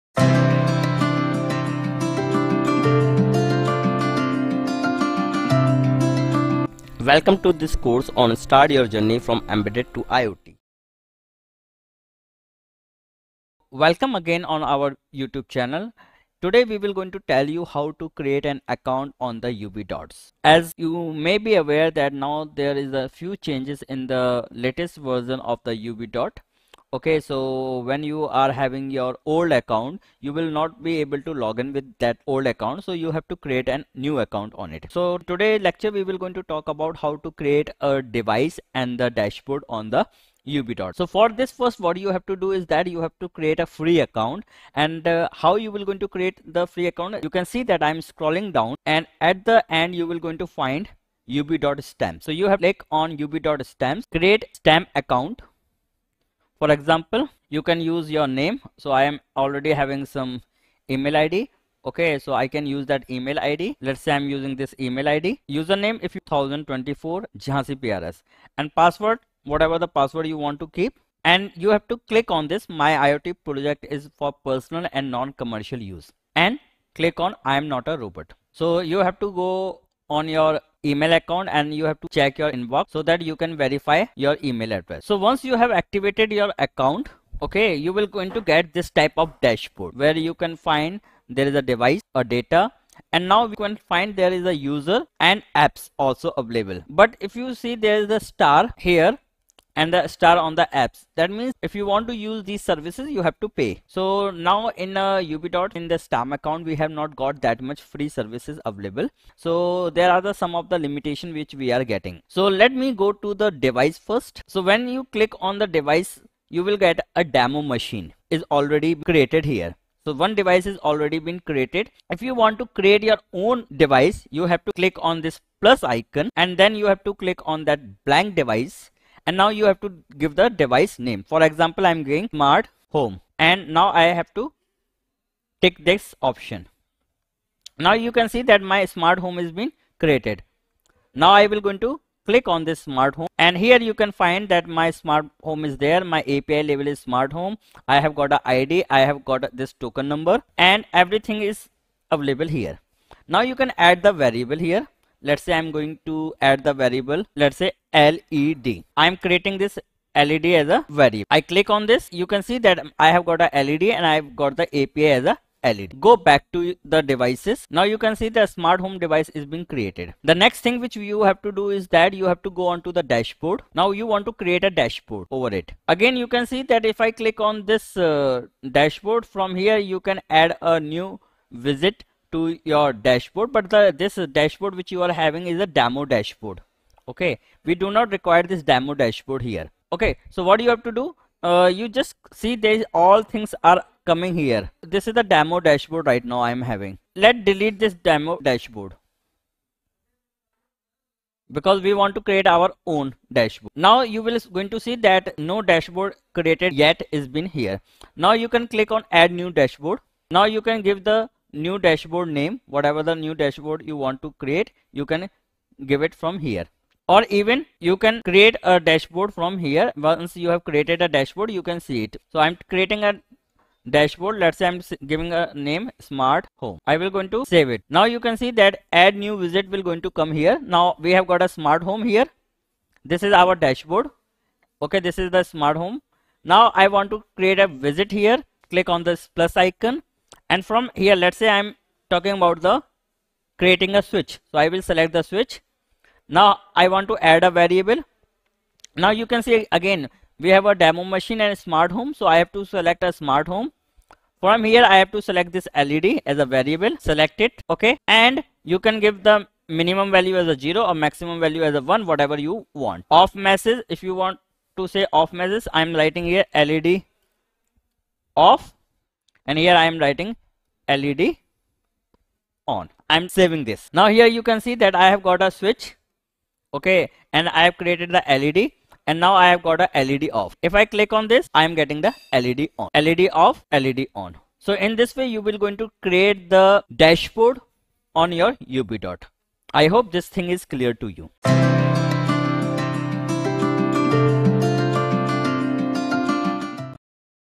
"Step-by-Step Guide: Building and Setting Up Device and Dashboard on Ubidots"
In this detailed step-by-step guide, we will walk you through the process of creating a device and dashboard on Ubidots, a powerful IoT platform. Whether you're a beginner or an experienced developer, this tutorial is designed to help you get started with building and setting up your own device and dashboard on Ubidots.

First, we will go over the fundamentals of Ubidots and how it can be used to monitor and control your IoT devices. We'll explore the various features and capabilities offered by Ubidots, allowing you to make informed decisions on how to best utilize this platform for your projects.

Next, we'll dive into the creation of devices on Ubidots. You'll learn how to define the properties and attributes of your devices and how to easily integrate them into your IoT network. We'll cover everything from naming your devices to setting up communication protocols and authentication methods.

Once your devices are set up, we'll guide you through the process of creating custom dashboards on Ubidots. You'll discover the vast array of tools and widgets available to visualize and interact with your IoT data. From widgets displaying real-time sensor readings to control buttons for managing your devices, Ubidots offers an intuitive dashboard builder to suit your specific needs.

Furthermore, we'll provide helpful tips and tricks to optimize your device and dashboard setup on Ubidots. We'll explore strategies to improve data accuracy, enhance security measures, and ensure seamless connectivity between your devices and the Ubidots cloud.

By the end of this tutorial, you'll have a solid understanding of how to create and configure devices, as well as design compelling dashboards on Ubidots. Whether you're building a smart home system or a complex industrial automation solution, this guide will equip you with the necessary knowledge to get up and running with Ubidots quickly and efficiently.

So, join us in this immersive journey of creating your own device and dashboard on Ubidots! Don't miss out on this valuable opportunity to learn and master the art of IoT with one of the leading platforms in the industry.